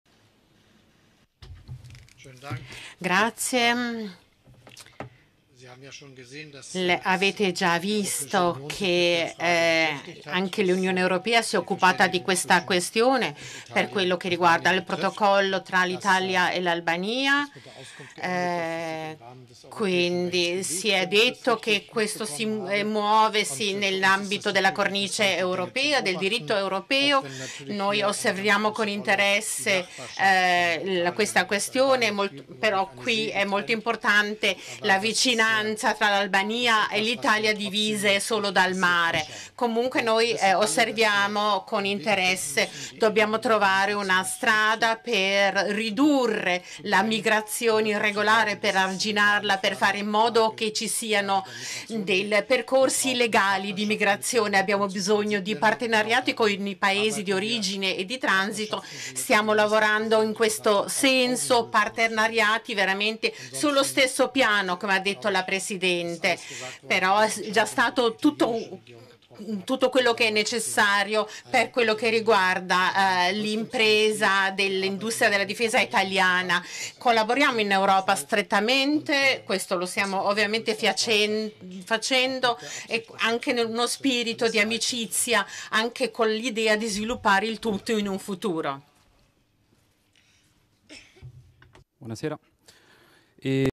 Scholz a Giorgia Meloni: "Ci interessa il vostro accordo con l'Albania sui migranti"
(Agenzia Vista) - Berlino, 22 Novembre 2023 - La Germania osserva "con interesse" la questione relativa al protocollo sui migranti tra Albania e Italia. Lo ha detto il cancelliere tedesco Olaf Scholz durante una conferenza stampa con la Presidente del Consiglio Giorgia Meloni in occasione del vertice intergovernativo in corso oggi a Berlino. "L'Unione Europea si è occupata della questione. Qui è importante la vicinanza tra Italia e Albania, divise solo dal mare. Dobbiamo trovare una strada per ridurre l'immigrazione illegale. Abbiamo bisogno di partenariati con i Paesi di origine e transito, e stiamo lavorando su questo piano. Collaboriamo strettamente in Europa, in uno spirito di amicizia", ha affermato il cancelliere tedesco. / Fonte video: Chigi